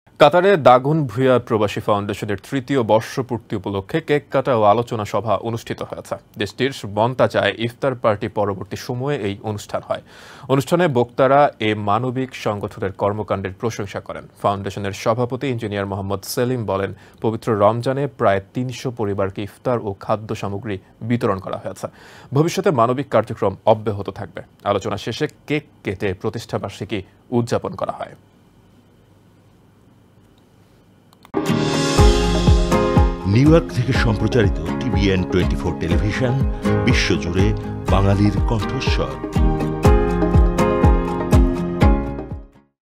কাতারে দাগনভূঞা প্রবাসী ফাউন্ডেশনের ইফতার পার্টি | TBN24 NEWS | Daganbhuyan Probashi Foundation
কাতারে দাগনভূঞা প্রবাসী ফাউন্ডেশনের তৃতীয় বর্ষপূর্তি উপলক্ষে কেককাটা ও আলোচনা সভা অনুষ্ঠিত হয়েছে। দেশটির মন্তাজায় ইফতার পার্টি পরবর্তী সময়ে এ অনুষ্ঠান হয়।